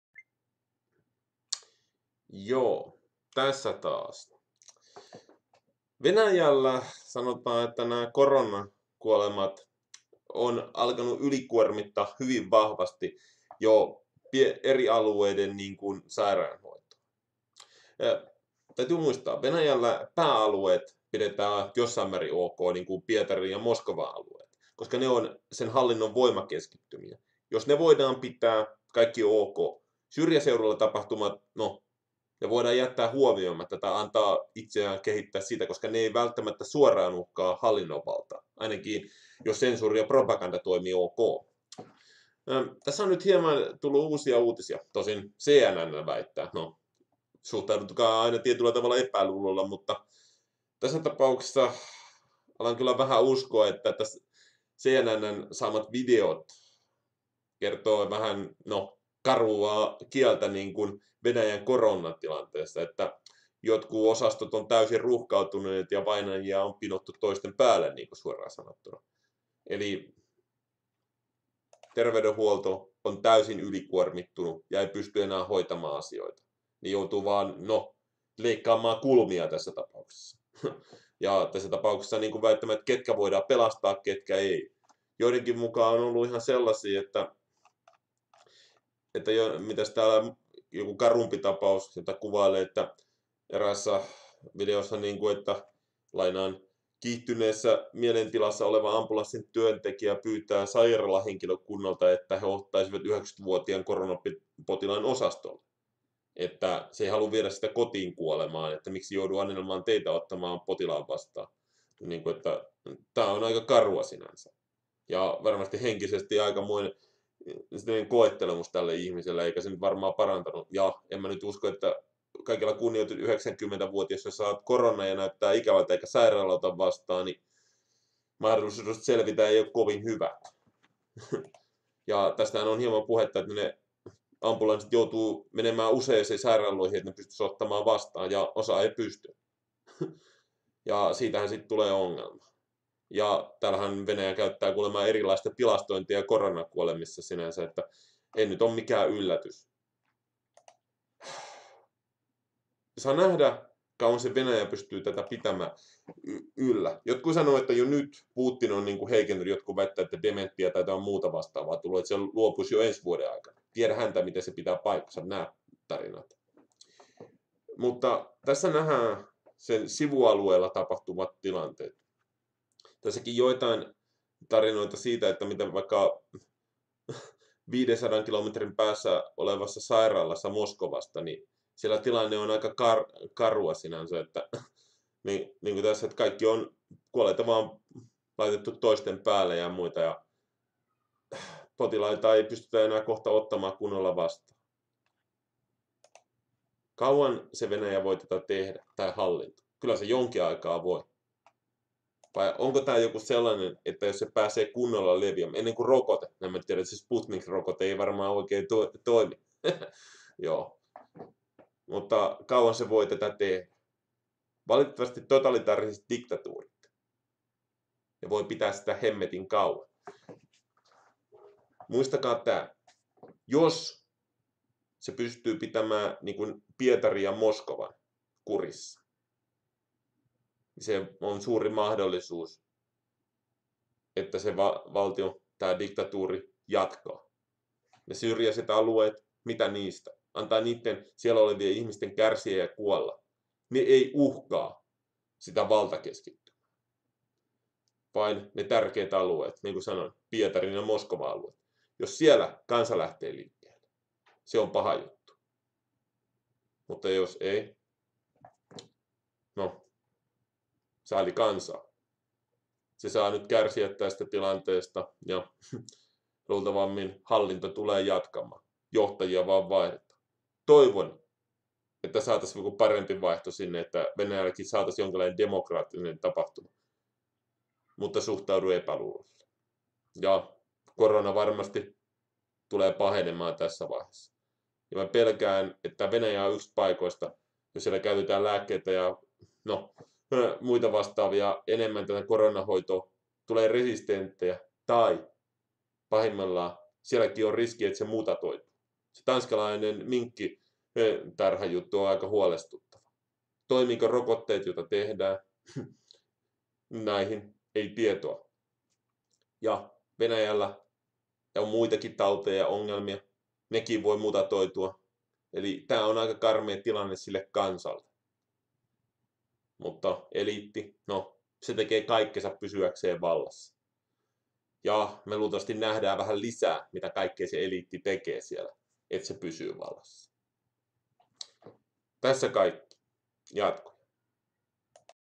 Venäjällä sairaalat ovat täynnä koronapotilaita?
Venäjältä on tullut esiin karumpia salaa kuvattuja videoita?  Koronapotilaita ja kuolleita on reilusti sairaaloissa ja tätä salataan?  Ikävä tilanne on että tähän on tuskin tulossa muutosta?